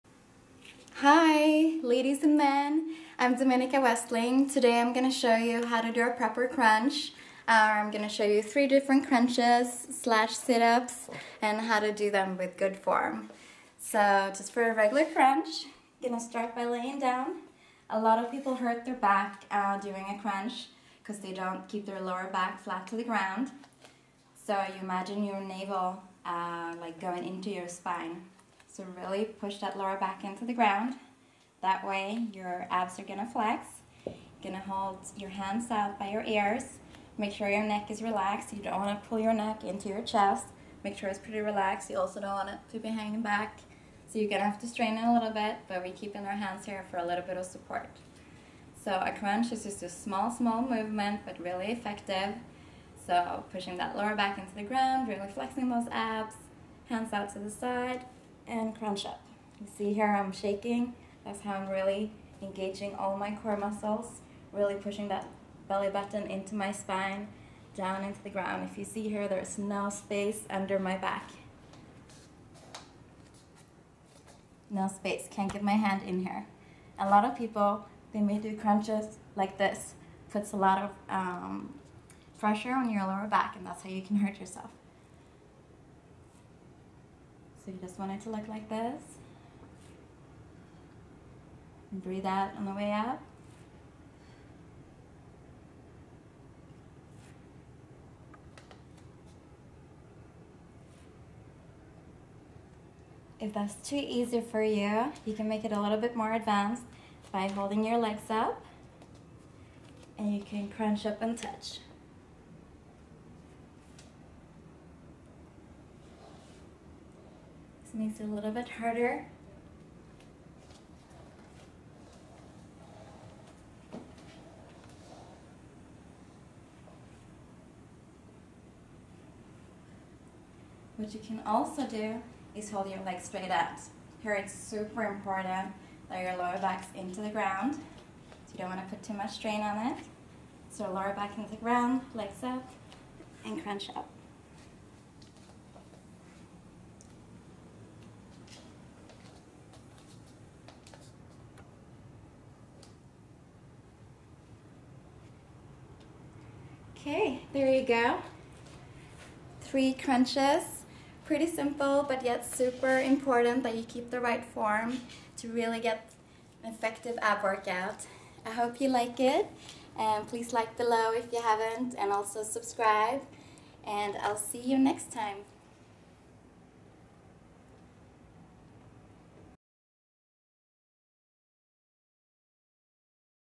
Crunches: How to do Crunches for Sexy Abs
Heres how to do crunches for sexy abs and a firm waist.
In this video model Dominica Westling is showing you several ways to do crunches to make it a great abdominal exercise for you. Give it a try.